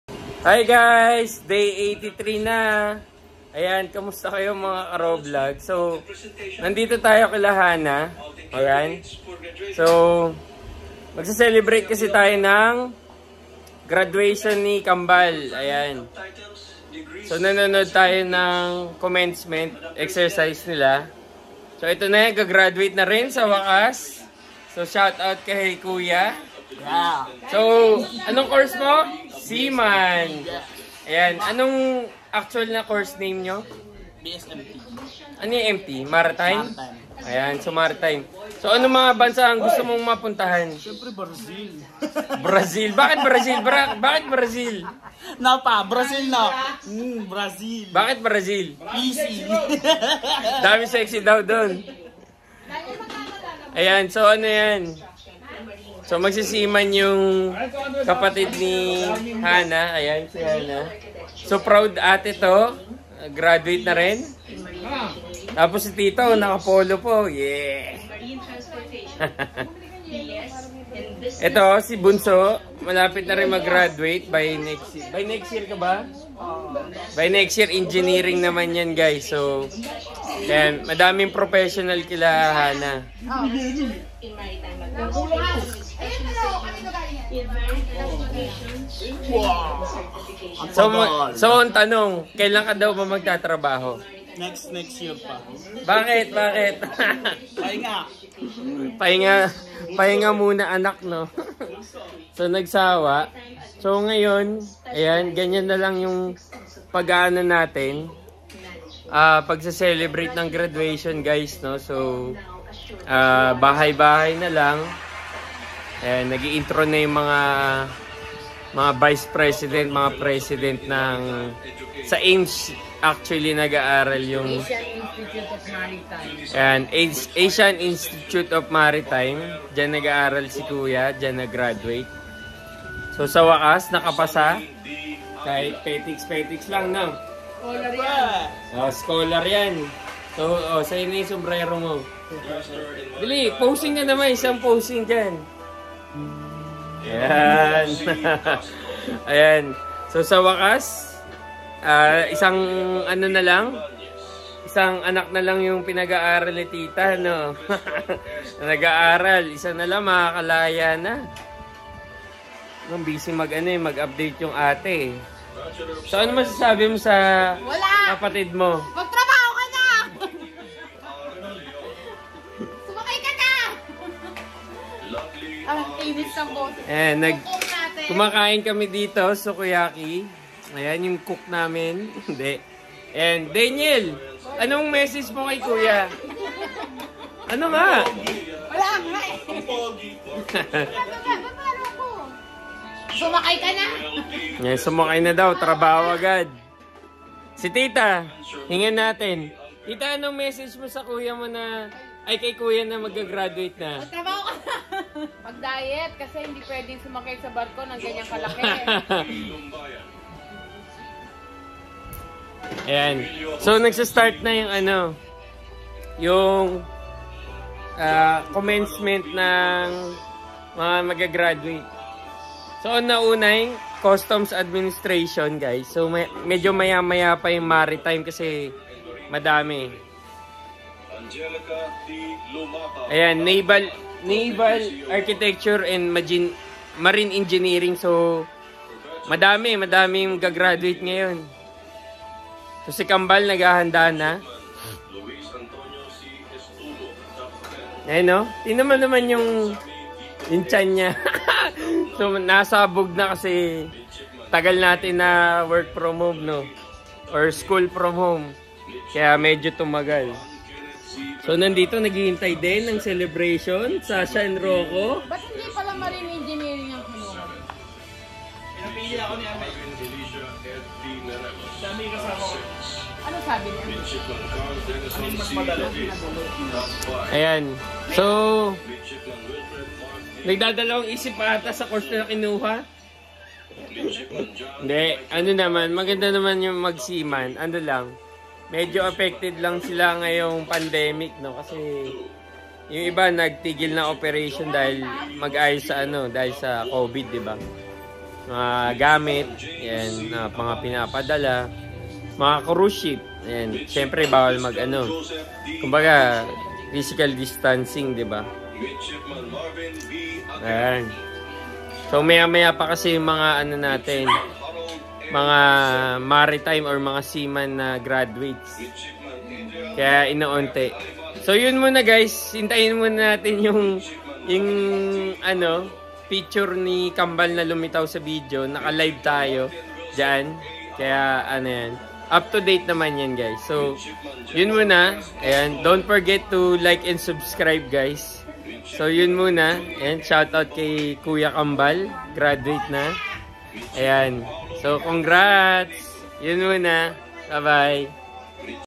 Day 83- Best Feeling in the world Graduation!!!
Congratulations to all Graduates today The 50th Commencement Exercises of Asian Institute of Maritime Studies.

Especially to Kuya Kambal for finally graduating after 3 Years in College so fast!!!

Proud of you and of course your Parents and Siblings for finally Graduating.